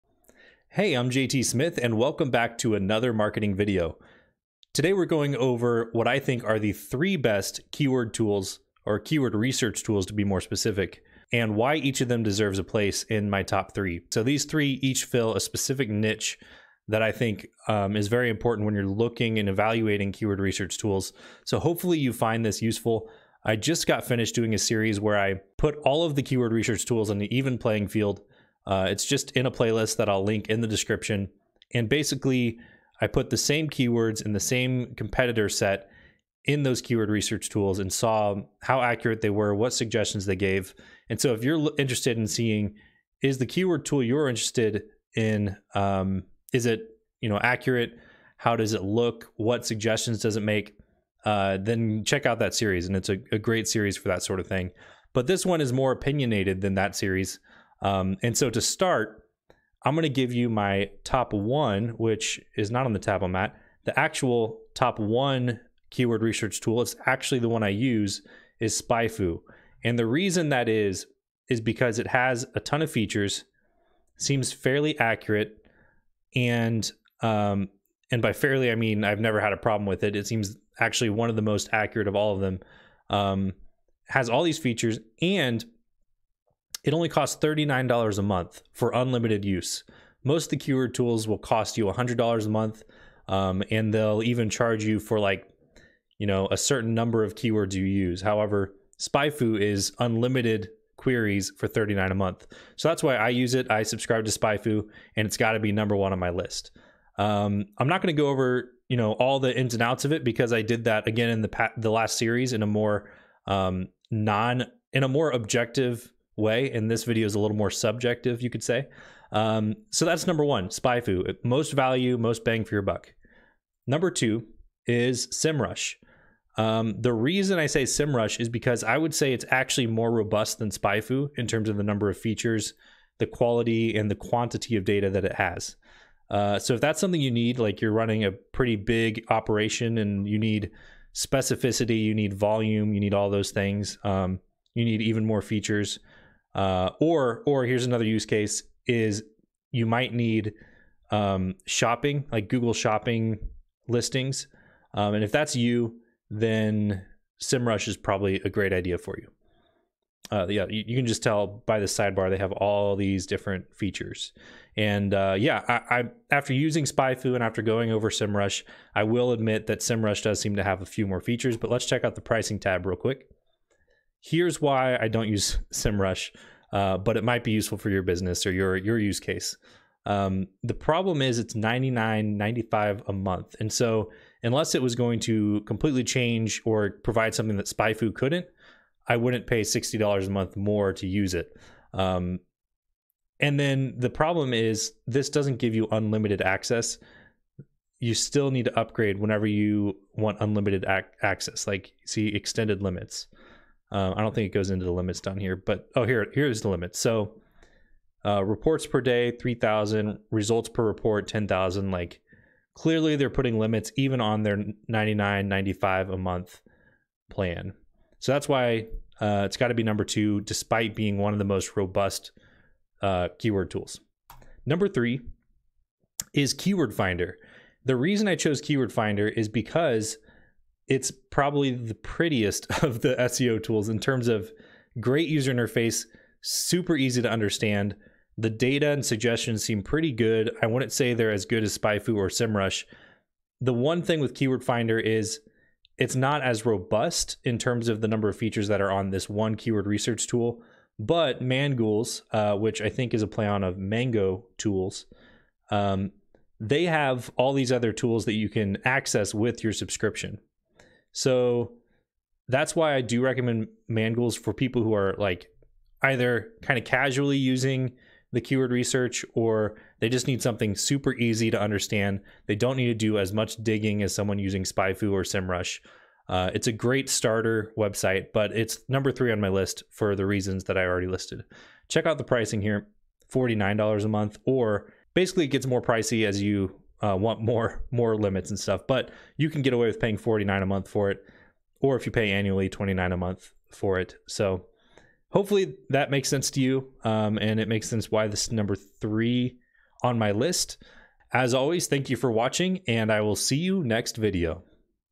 3 Best Keyword Research Tools - Value vs Robust vs User Friendly
The best keyword research tools have some combination of value, robustness, and user friendliness.

If you want to see a demo of these platforms, as well as many other keyword research tools, the following playlist contains a comparison of each one. I use the same keyword and competitor for a fair test.

After doing the above test, I was able to determine which keyword research tool I think is the best one.

In terms of value, you need a keyword tool that does all of the main functions with accuracy, but also has a low price. It needs to be fairly user friendly, and great for the general audience of marketers to use. That's why I picked SpyFu for best overall keyword research tool.

Now if you want to use a more robust and probably more accurate tool, SEMRush is there for you! It's got an amazingly large index of search data so you can slice and dice keywords and competitive insight however you want.

Finally, I tossed KWFinder by Mangools in there because it is definitely the easiest to use and has the most modern, up-to-date interface. I could see them developing more features in the future without tarnishing the wonderful interface they have now.

The Marketing Tools I Use to Get an Advantage